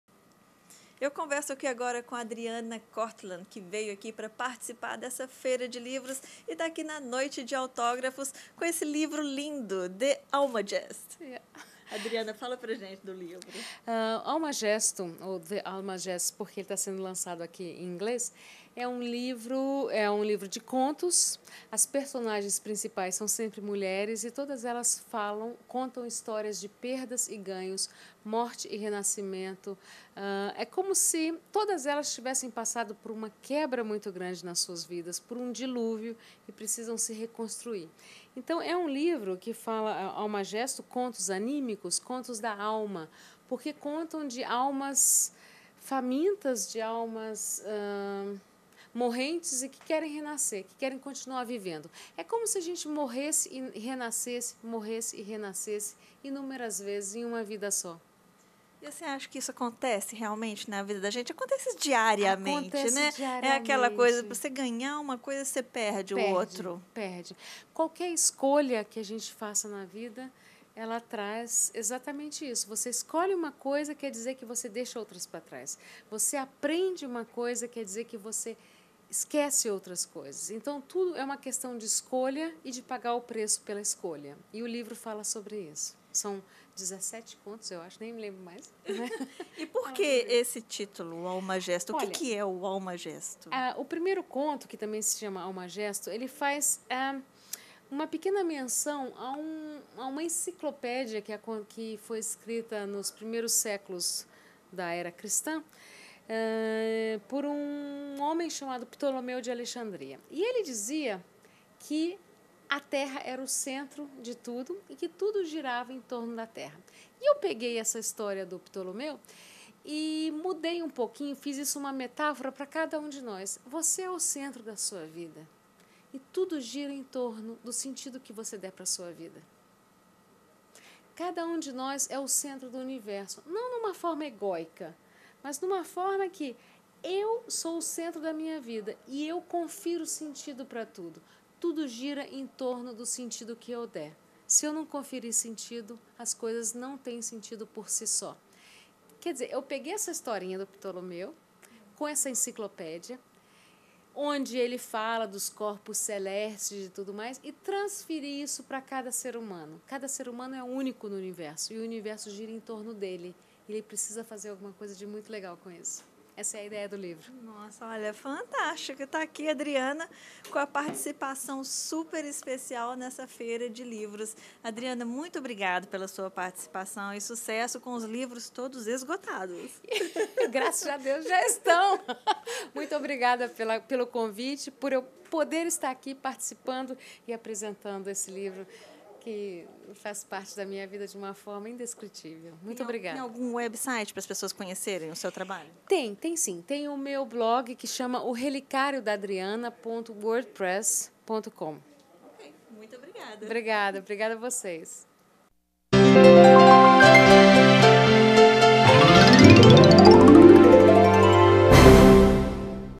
Adriana Kortlandt - The Almagest
Cristina Natividade entrevista a escritora e autora do livro The Almagest em Nova York.

Assista esse e outros videos no site oficial do Vejatv no endereço